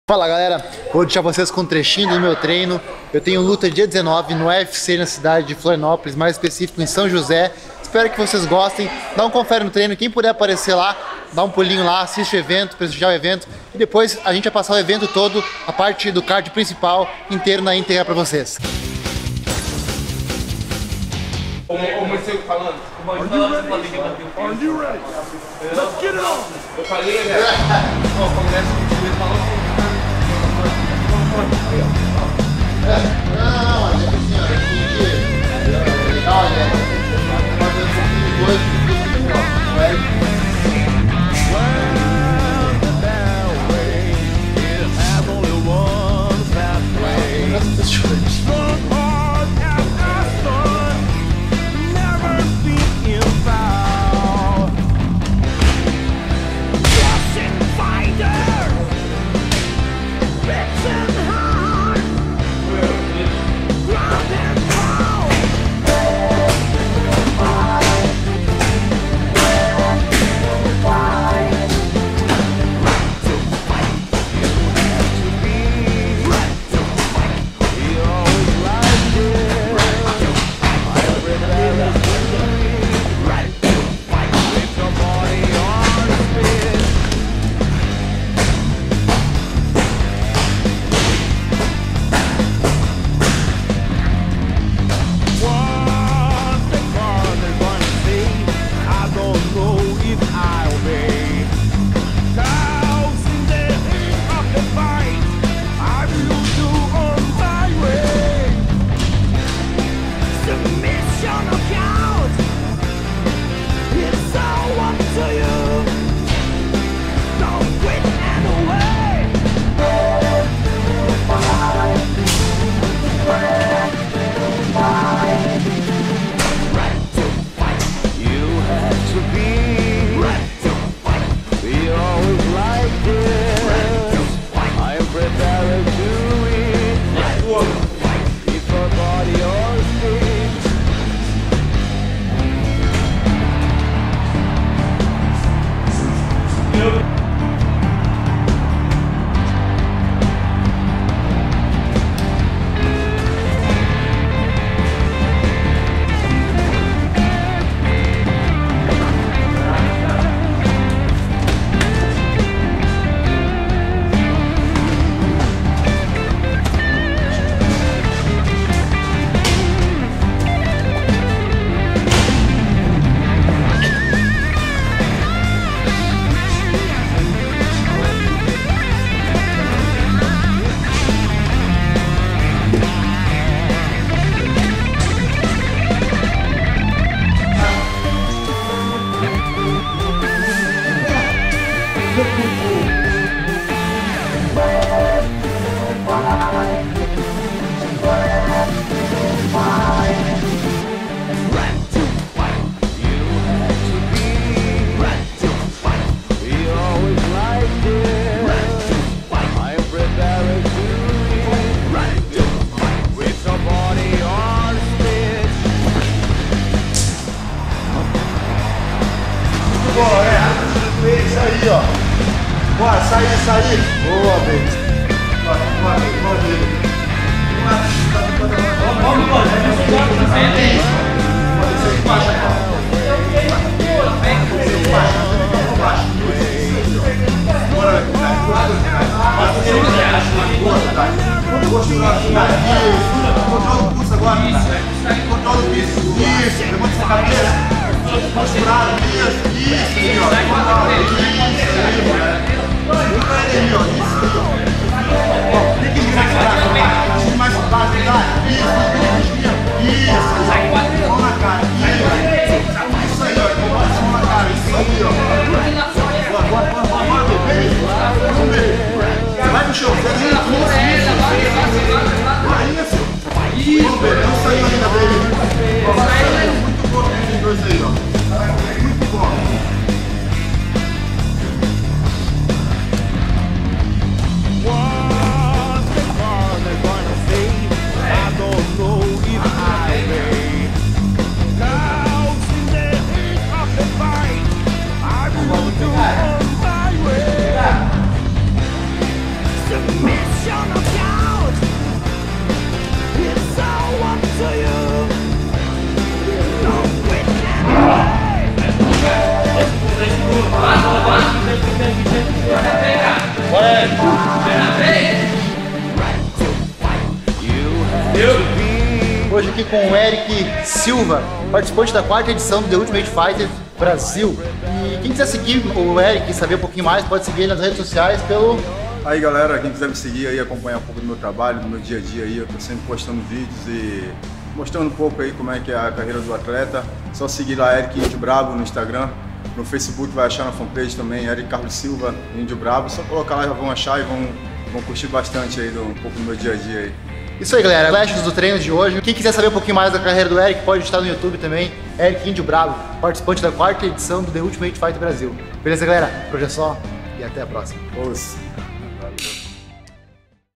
Treino Pré Luta AFC 48
Treino pré luta para o evento Aspera FC 48 que ocorre dia 19/11/2016 em São José - SC